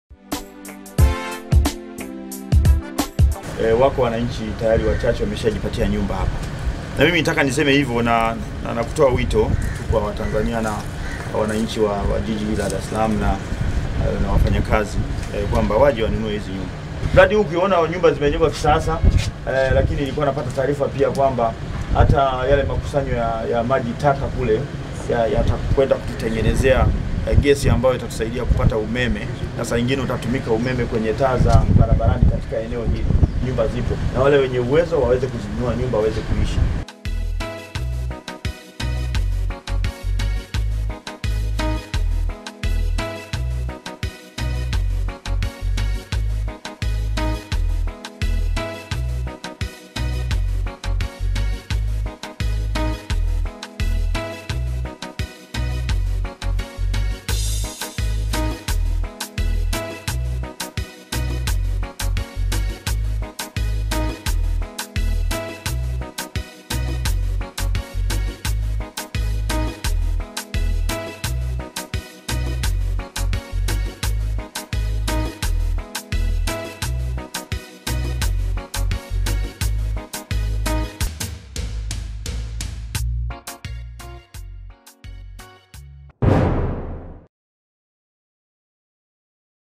Watumishi Washauriwa kununua nyumba za TBA
Naibu Waziri wa Ujenzi, Uchukuzi na Mawasiliano, Mhe.  Elias Kwandikwa ashauri  Watumishi kununua Nyumba za TBA  za Bunju - Dar es Salaam